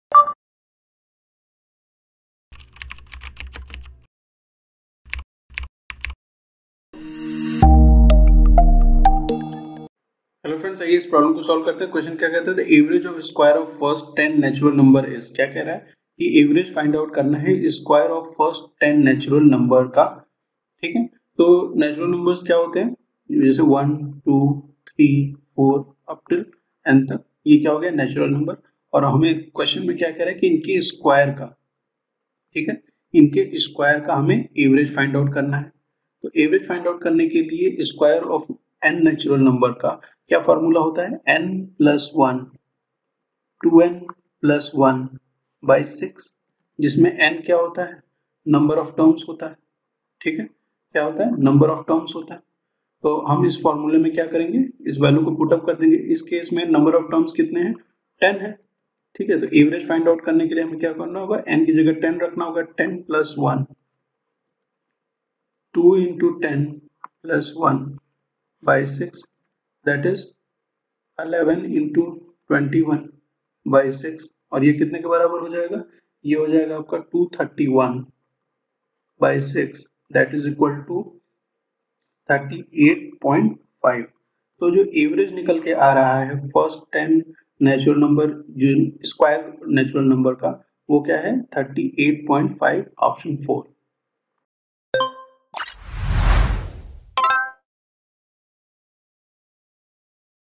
The average of the squares of first ten natural numbers is (1) 35.5 (2) 36(3) 37.5 (4) 38.5 ?
The average of the squares of first ten natural numbers is